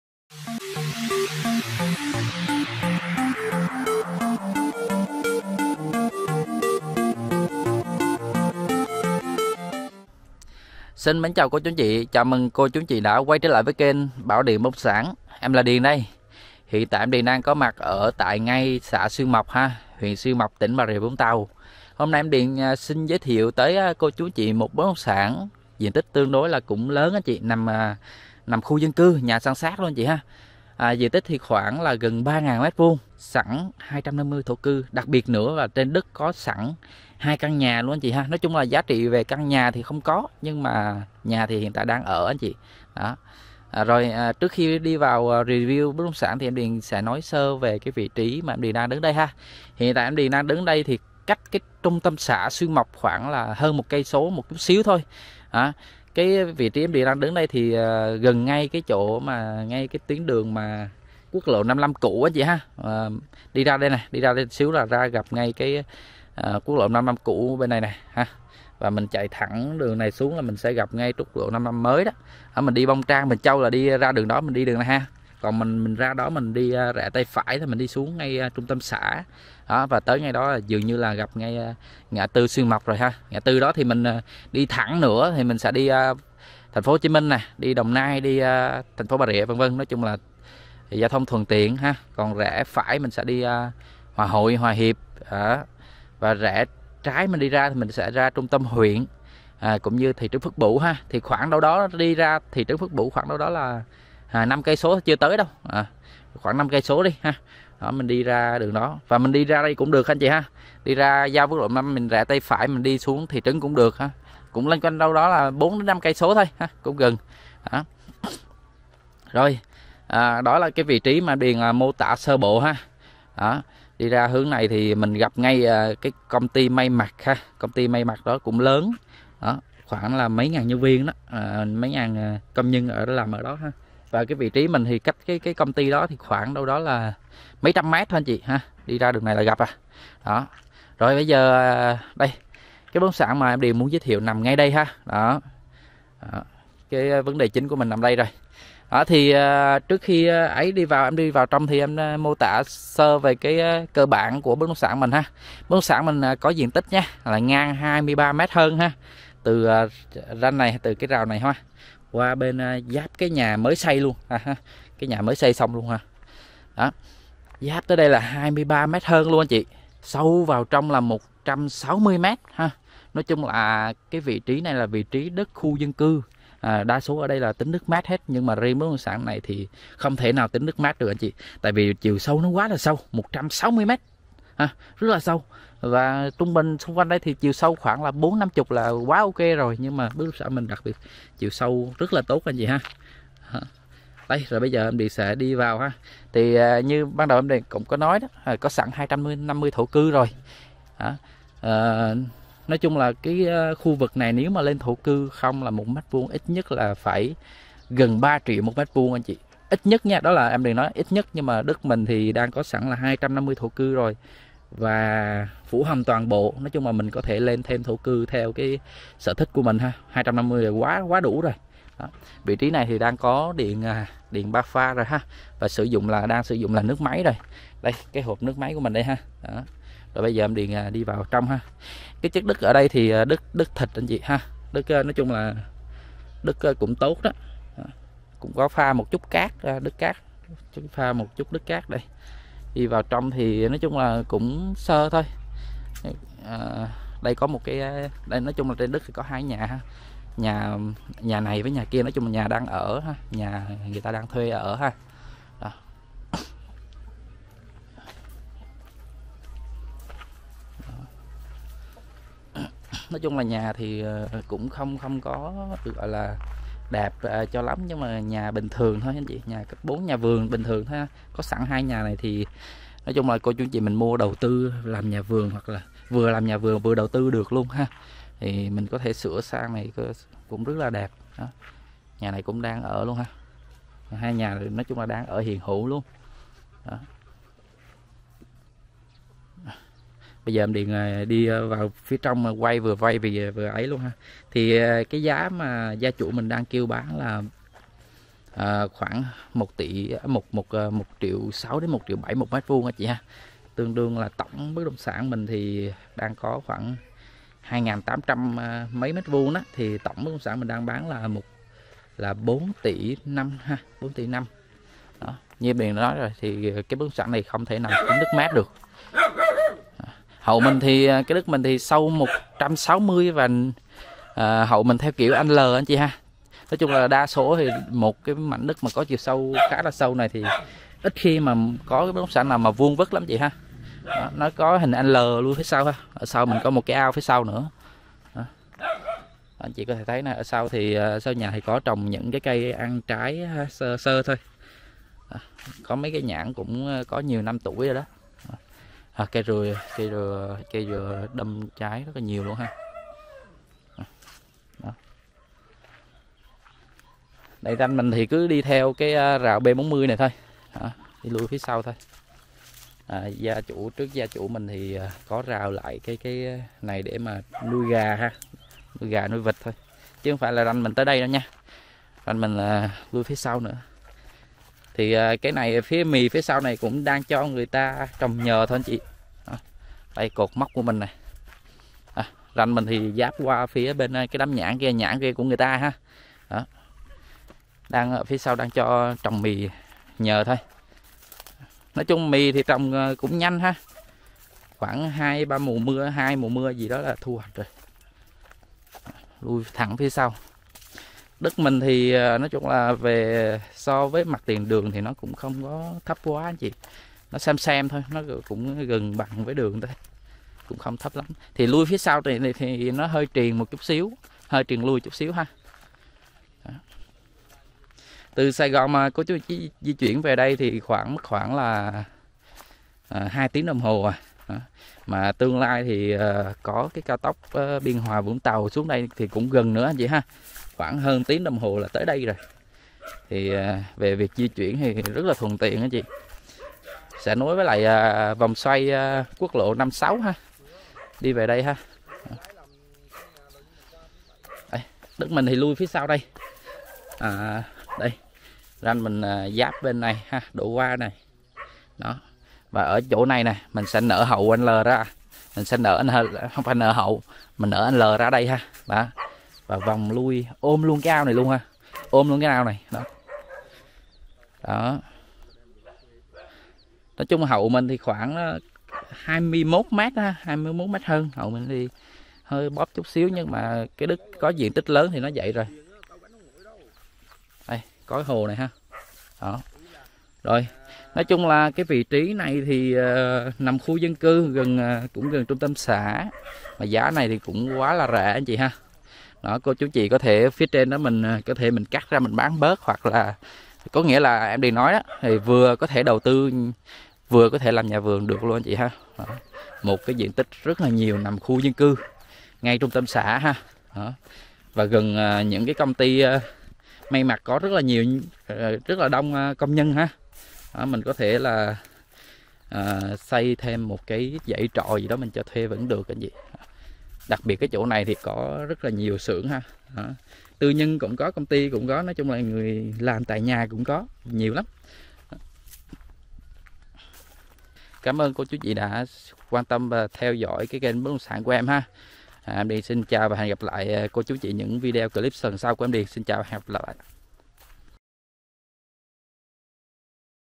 Bán đất vườn nằm giữa lòng khu dân cư tại Xuyên Mộc Giá ≈ 1,6 tr/m2 | LH 0788668834 (Điền)
* Tọa lạc : xã Xuyên Mộc , Huyện Xuyên Mộc , Tỉnh BRVT
👉 Diện tích : 24x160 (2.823m2)   👉 Thổ cư :   250 thổ cư
👉 Pháp lí : Sổ Riêng  👉 Hướng : Nam
🏁Giá chỉ : 4 tỷ 500 có thương lượng
 💯 ĐIỀN VẪN CÓ VÀI LÔ CHỦ GỬI BÁN GIÁ HỢP LÍ MỜI CÔ CHÚ A/C XEM THAM KHẢO THÊM !

🌈 Đặc biệt tại Huyện Xuyên Mộc có rất nhiều khu du lịch,Dự án,Resort nổi tiếng như: Biển Hồ Tràm, suối nước nóng Bình Châu, Novaworl, Meliã, Cầu ngắm biển Hamptons Pier, Sanctuary Ho Tram, v.v..